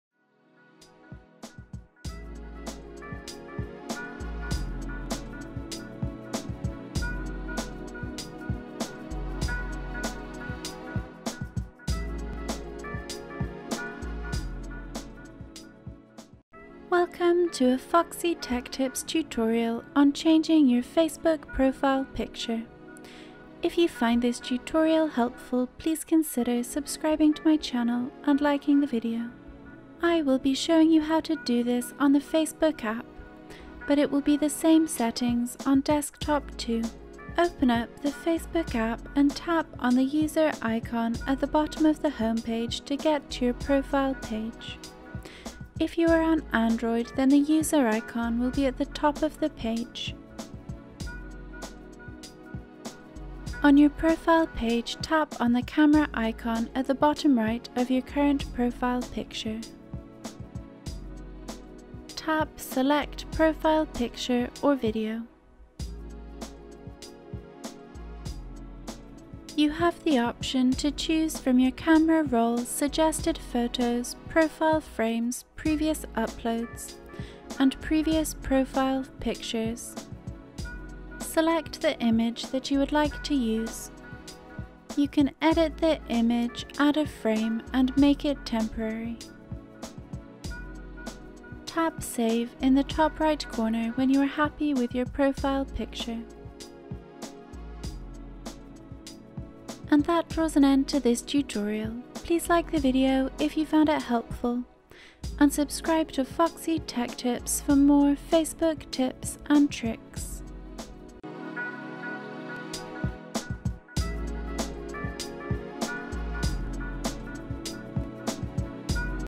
How To Change Facebook Profile Picture
Easy to follow tutorial on changing your profile image on your Facebook profile! Learn how to set a different picture as your profile picture on Facebook the Facebook mobile app now!

Steps:

-  will be showing you how to do this on the Facebook app, but it will be the same settings on desktop too.

- Open up the Facebook app and tap on the user icon at the bottom of the homepage to get to your profile page. If you are on android then the user icon will be at the top of the page.

- On your profile page tap on the camera icon on the bottom right of your current profile picture.

- Tap ‘select profile picture or video’

- You have the option to choose from your camera roll, suggested photos, profile frames, previous uploads, and previous profile pictures.

- Select the image that you would like to use.

- You can edit the image, add a frame, and make it temporary.

- Tap ‘Save’ in the top right corner when you are happy with your profile picture.